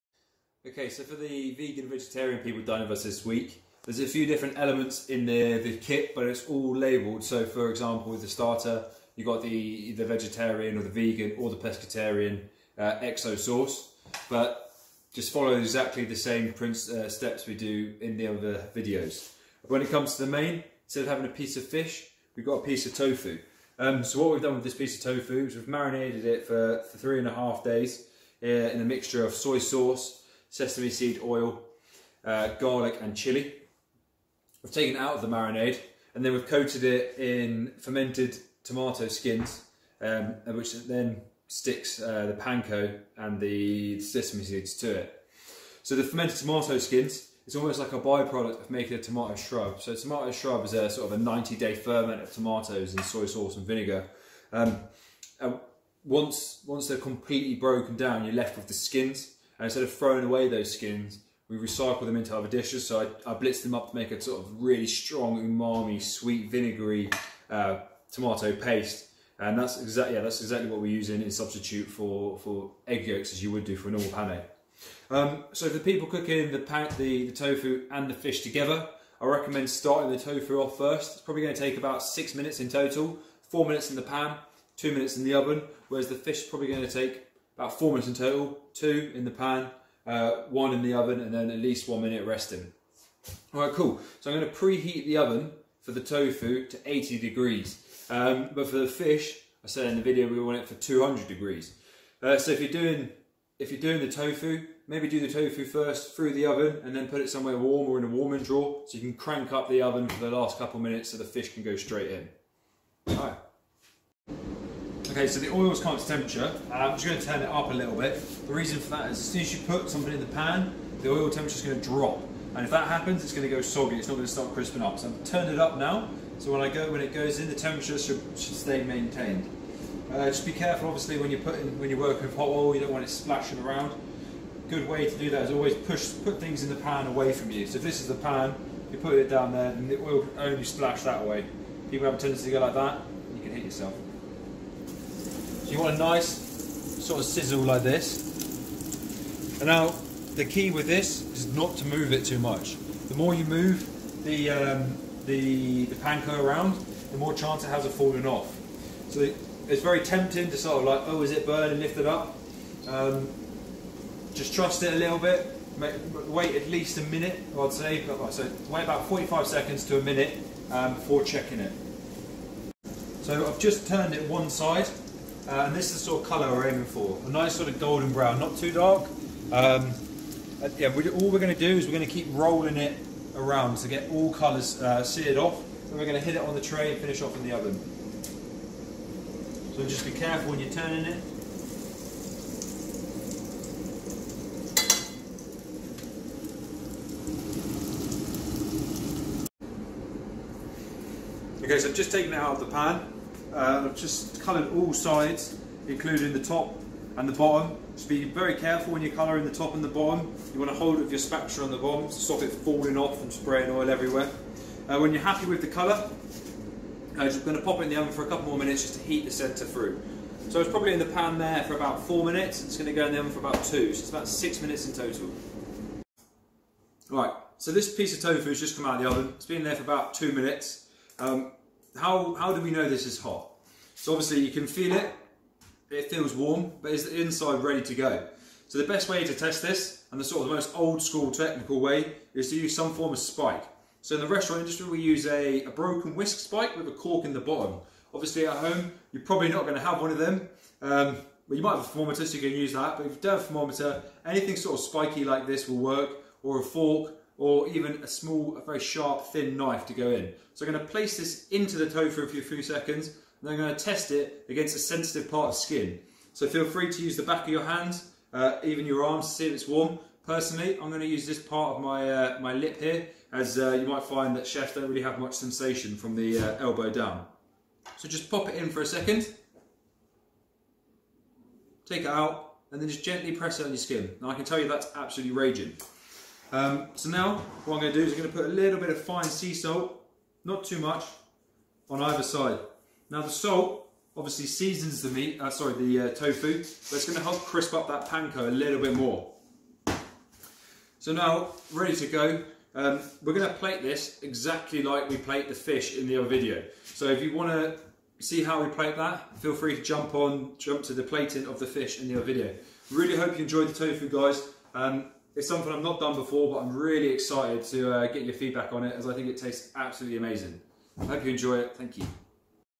Vegan/Vegetarian 04.07.20
For those of you who are vegan or vegetarian and are dining with us this week, take a look at how to cook your tofu!